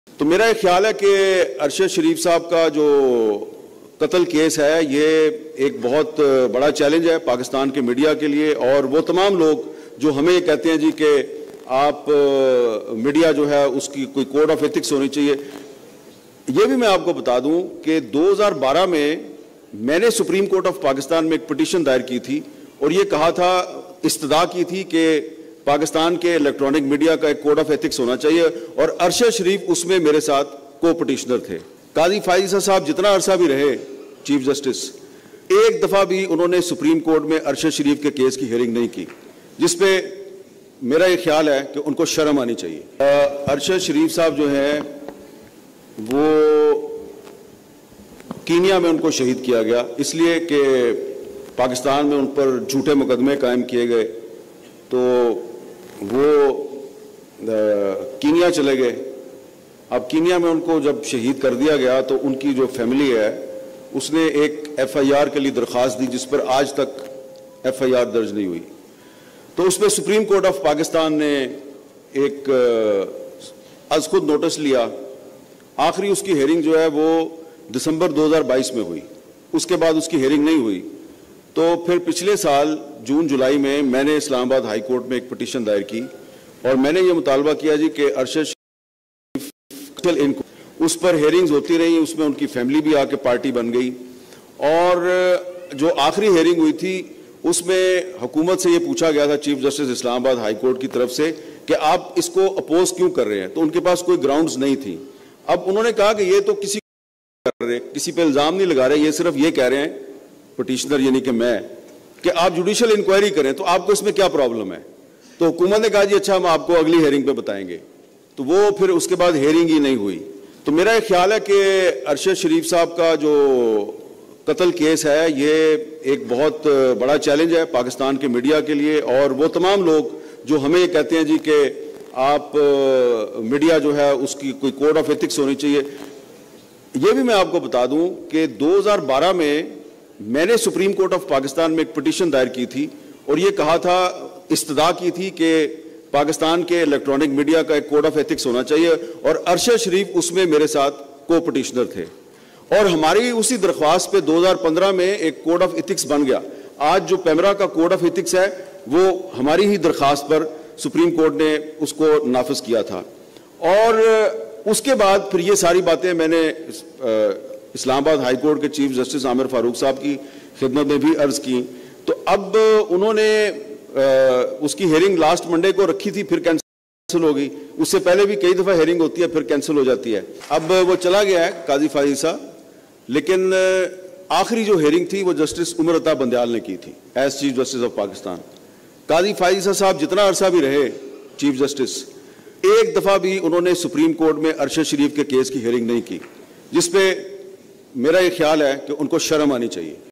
🔥 What Happened to Arshad Sharif? | Hamid Mir's Fiery Speech | Must Watch! 🎤
📢 Renowned journalist Hamid Mir delivers a fiery speech shedding light on the tragic events surrounding Arshad Sharif. What really happened, and how does it connect to the current political climate in Pakistan? 🇵🇰

🔍 Hamid Mir's bold words raise serious questions and reveal insights that you can’t afford to miss.